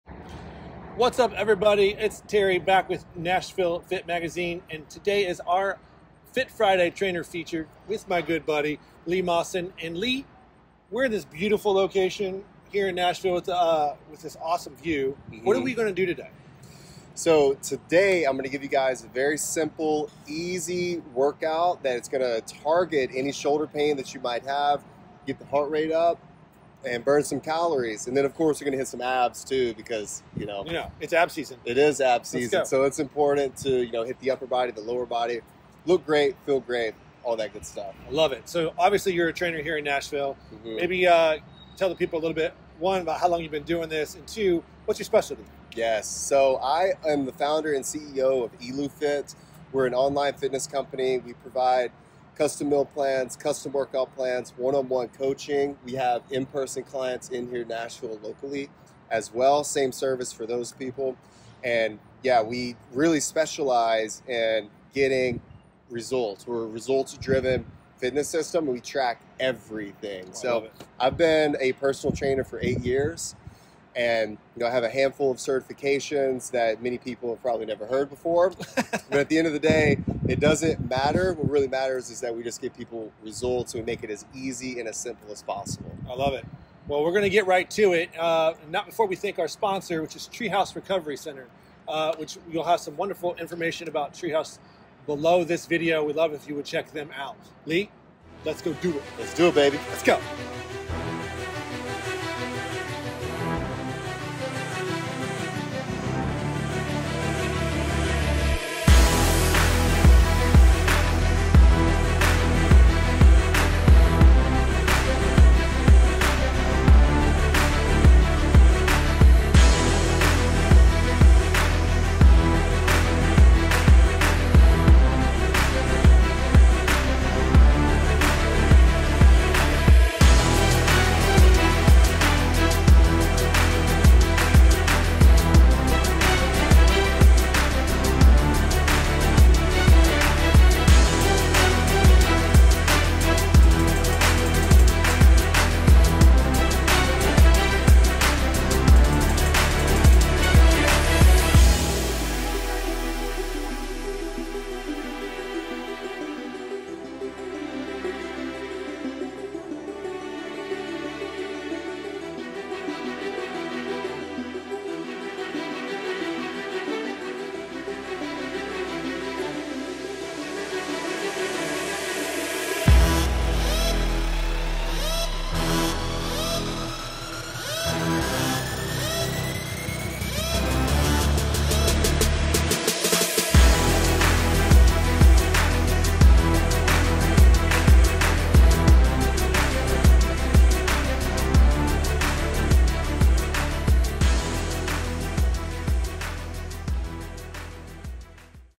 Fit Friday Trainer Feature w/ Lee Maasen
Lee Maasen is an amazing local personal trainer who has made a career of helping people gain amazing strength as well as lose weight.
Here's the workout

A1: Above Support Hold - 3 x 15s hold

A2: Jump Split Squat - 3 x 10r/each

A3: Crunch - 3 x 15-30r

B1: Chest To Wall Hold - 3 x 30s

B2: Inverted Row - 3 x 10r

B3: Sissy Squat - 3 x 10r